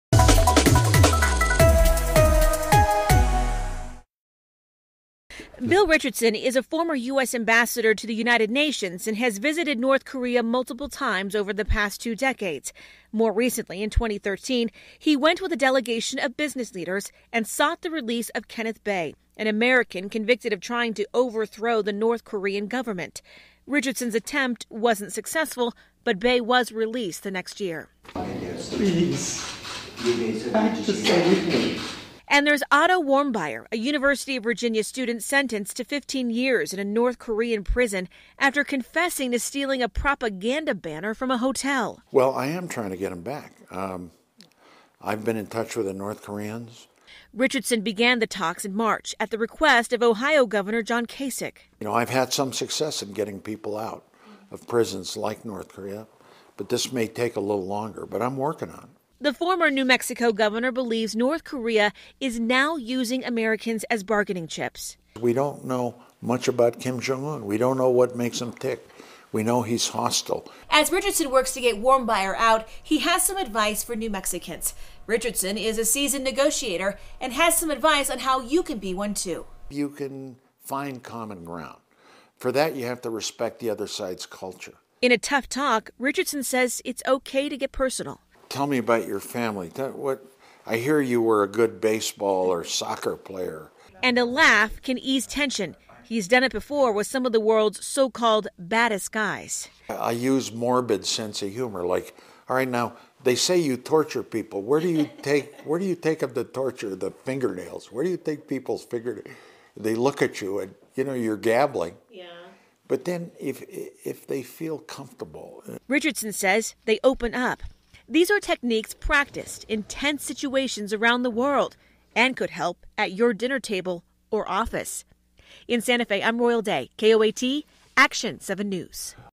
Bill Richardson Talks Diplomacy
Bill Richardson has negotiated numerous times with North Korea.